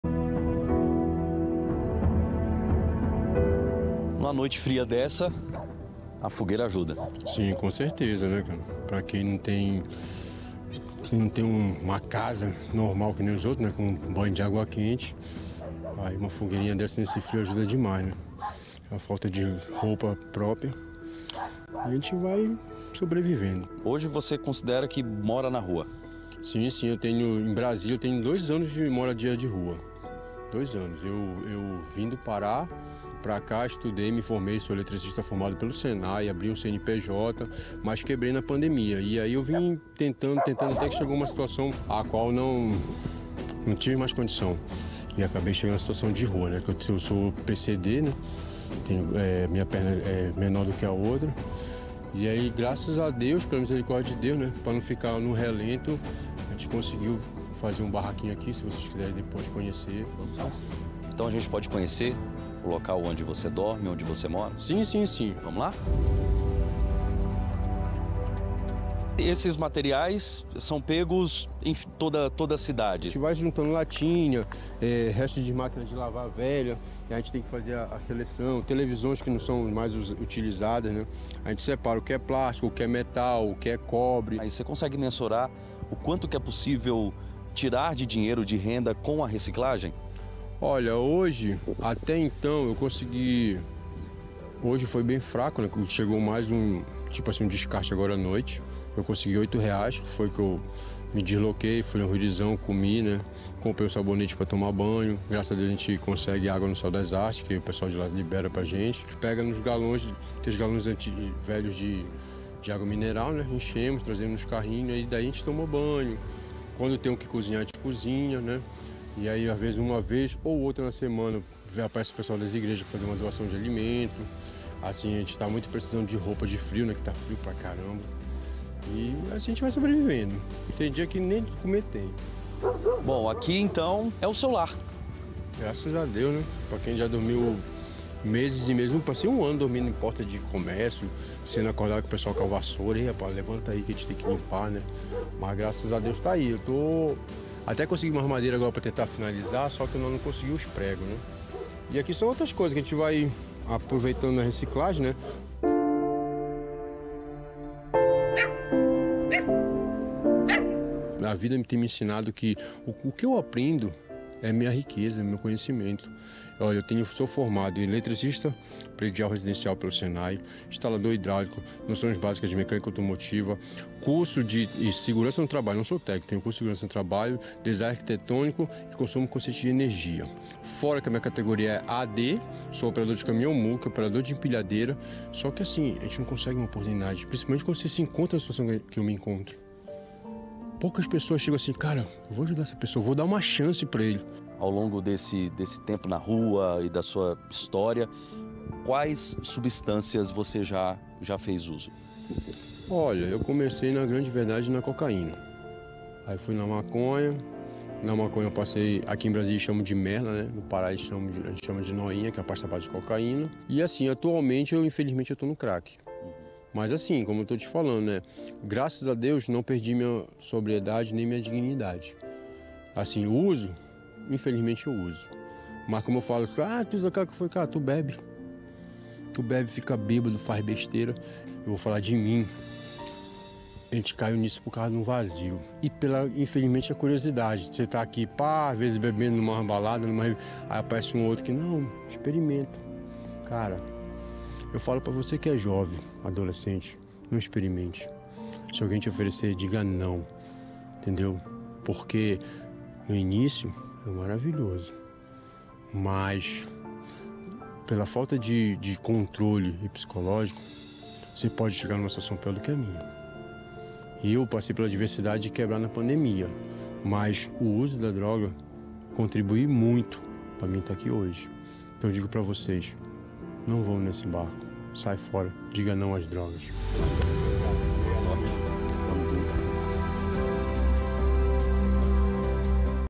DF ALERTA - A dura realidade de um trabalhador em situação de rua
19/07 - Josué é eletricista, parou de trabalhar na pandemia e tenta se reerguer.